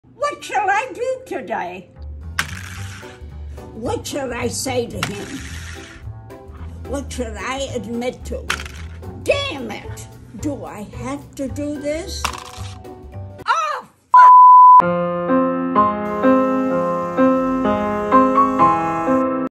Spinning The Plate Of Fate | Ross Smith
Granny spins the plate of fate, see what the plates tell her to do! The results could be amazing, but they might be awful too.